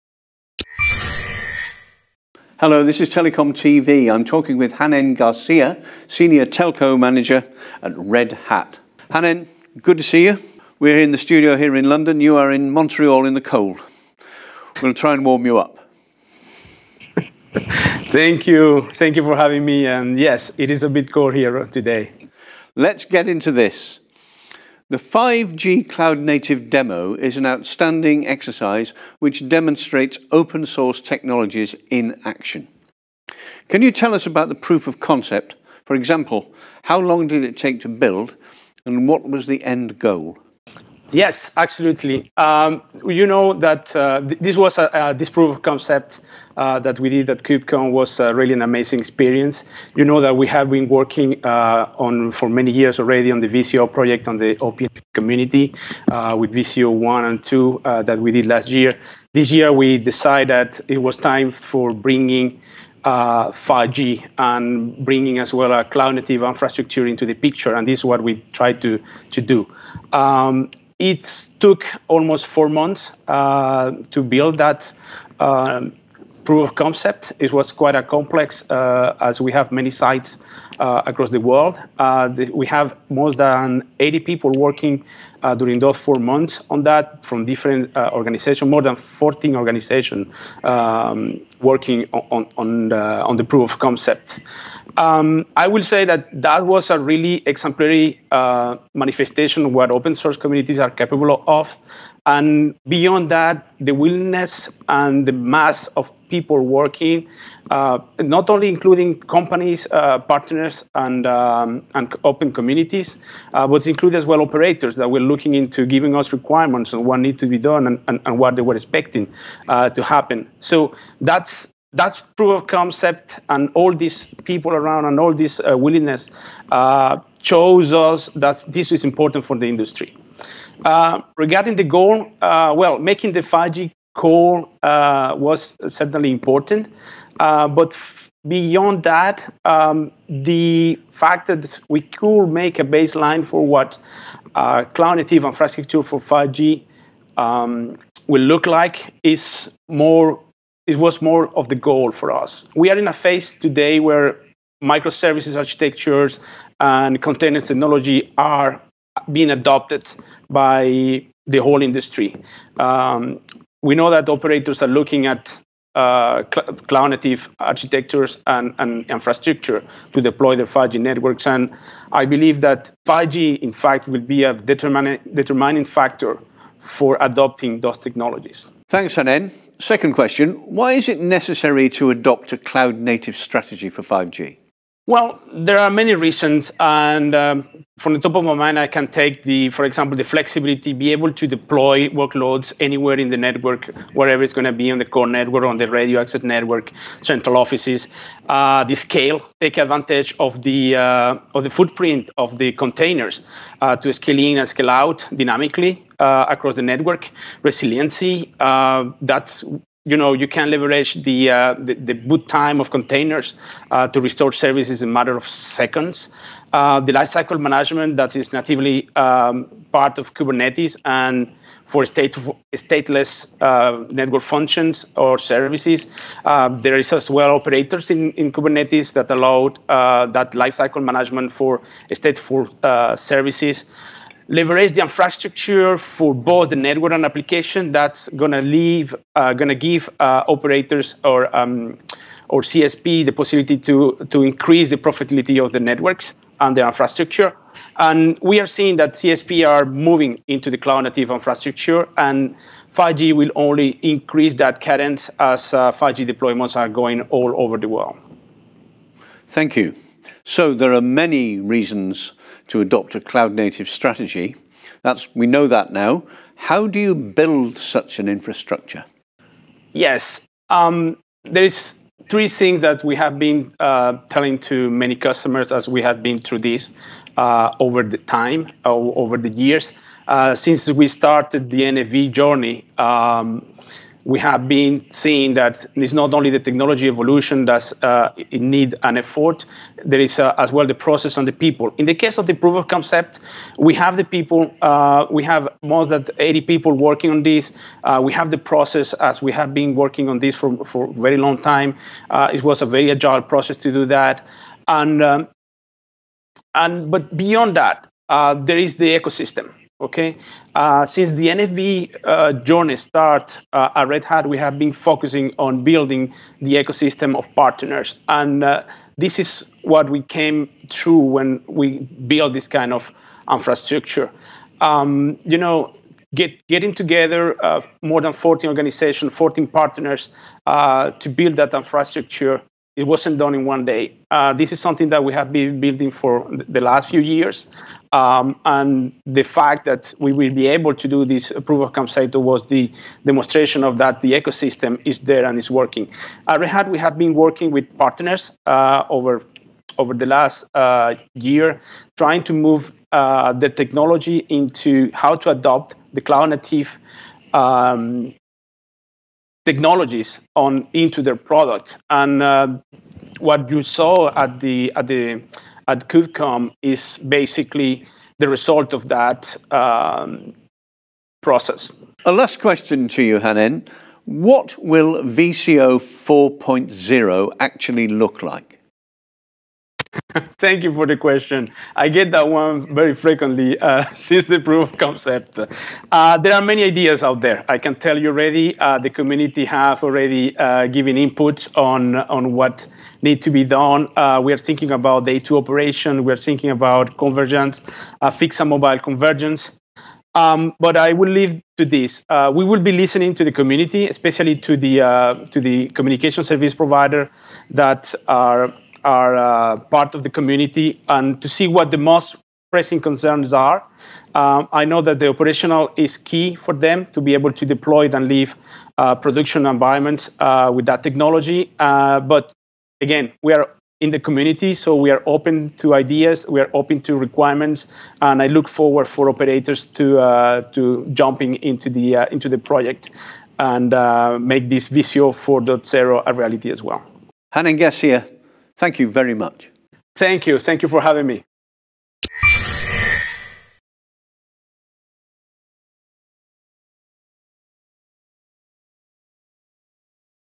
Rome wasn't built in a day. Neither was the E2E 5G Cloud Native Network live demo
Late last year at KubeCon + CloudNativeCon North America, a team of more than 80 volunteers from 14 companies and organisations together with six open source communities and standards groups unveiled their E2E 5G Cloud Native Network live demonstration from the VCO 3.0 (virtual Central Office) working group. The demo took a cloud native approach to building a 5G network prototype on open source infrastructure, including kubernetes, and was hosted and ran from Sophia Antipolis in France and Montreal, in Quebec, Canada and from a private cloud and the public cloud.

As Hanen Garcia of Red Hat explains, the demonstration was a great example of the power of partnership with Open Source communities, including companies, manufacturers, vendors and operators all working together in community to show a fully-functional, live proof of concept (PoC). It was a complex task that took four months of intense collaboration between groups and communities around the world and, Hanen Garcia says, the success of the exercise provided irrefutable evidence that the PoC was of great importance to the 5G industry in general and CSPs in particular because many operators had fed their requirements and expectations into the communities and other players who designed the PoC from the ground up.

The prime goal of the exercise (apart from the live demonstration of course) was to provide a technical baseline for what an operational, commercial 5G network will look and be like. Today, micro-service architectures and containers technologies are being adopted across the entire global industry as operators and CSPs examine cloud native architectures and infrastructure to deploy 5G networks. Indeed, as Hanen Garcia says, 5G will be THE determining factor in the adoption of micro-services and containers technologies.

Hanen Garcia explains that there were many reasons behind the decision to adopt a cloud native strategy for the 5G demo, including the innate flexibility to deploy workloads anywhere in the network regardless whether that be in the core, in the RAN or in central offices. Equally important is the ability to take advantage of the footprint of containers dynamically to scale-in and scale-out across the network. And then there is the life-cycle management that is a native component of kubernetes for stateless network functions and services.

As Hanen Garcia explains, "Leveraging the infrastructure for both networks and applications will give CSPs the chance to increase the profitability of networks and infrastructure and 5G will help that as 5G deployments spread worldwide. He adds that Red Hat has focussed on building an ecosystem of collaborative partners and the wisdom of that approach was vindicated in the coming together of members who deigned, developed and produced the world's first 5G Cloud Native Network live demonstration.

Featuring: Hanen Garcia, Global Telco Solutions Manager, Red Hat

Filmed at: TelecomTV Studios, London, December 2019